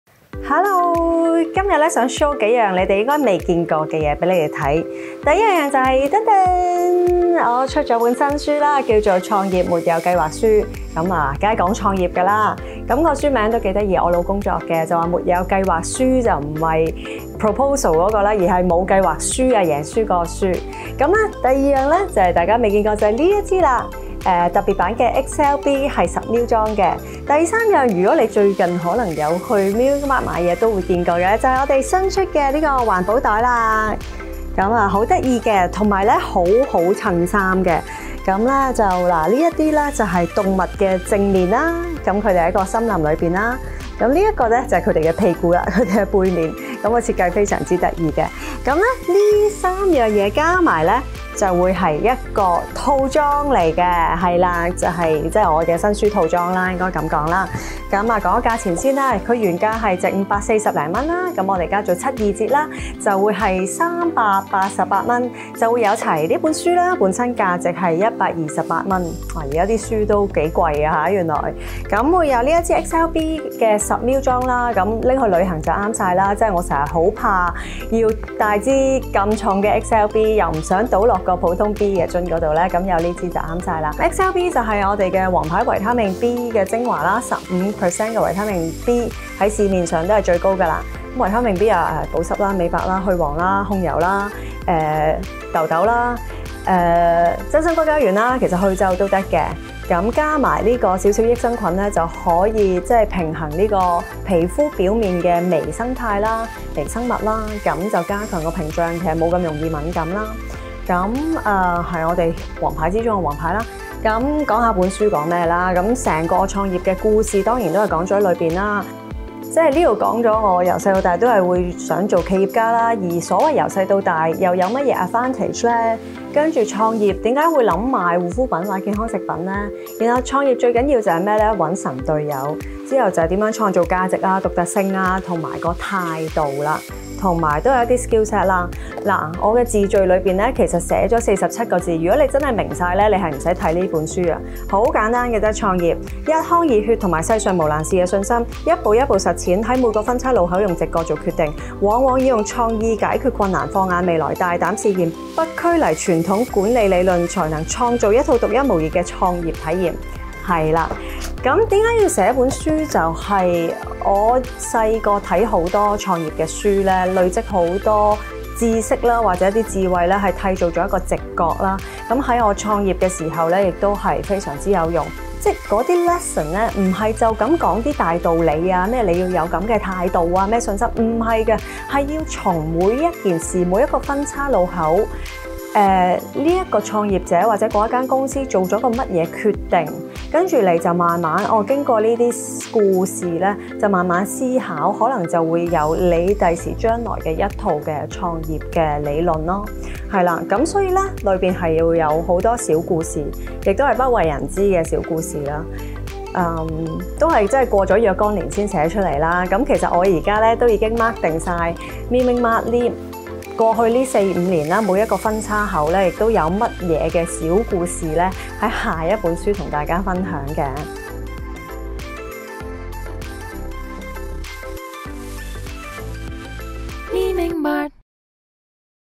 Erica限量新書套裝 x 創業沒有計劃輸
【Erica親筆簽名新書😻好！想！要！啊！】
想窺探Erica創業嘅心路歷程、經營生意嘅獨門小貼士及作為一個boss應具備嘅心態？您嘅機會黎啦😚😚Erica新書「創業沒有計劃輸」現已隆重推出❣️
本書將與你分享Erica奇妙的創業旅程，大談作為創業者需具備的獨有「貼地」質素，以及創業避雷之法，令你將可取之處套用到屬於自家嘅創業路💕～

書入面仲有不為人知嘅MI MING MART小故事😏，相信大家一定會喜歡🤗

MI MING MART x Erica Key to Success Set
❤️Erica😍親筆簽名😍新書「創業沒有計劃輸」
❤️SYNERGIE SKIN甦活原生B3昇華素 10ml [限量版😱]
❤️MI MING MART 靚靚環保袋

立即Follow MI MING MART IG | 留意最新動向